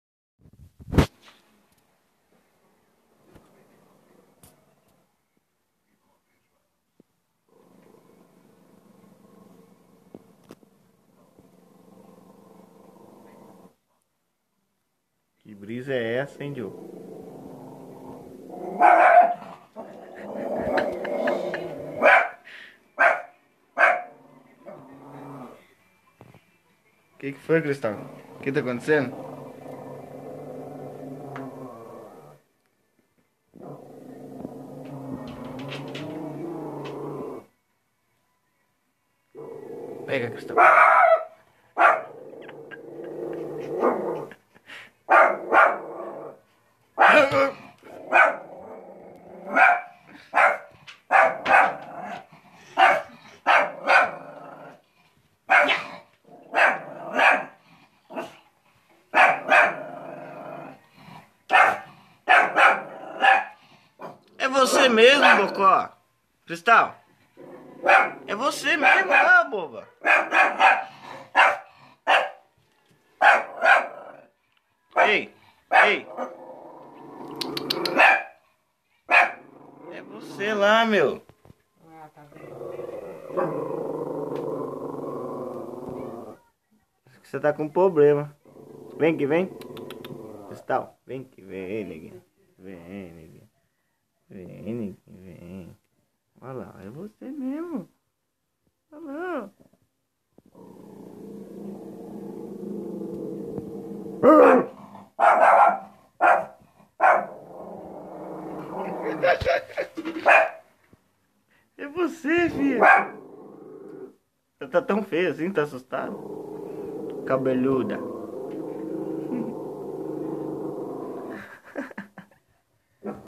comédia Cristal cachorra doida (shih-tzu )
Nem minha shih-tzu aguenta mais ficar em casa.. ta ficando doida !!

Nome: Cristal
Raça : shih-tzu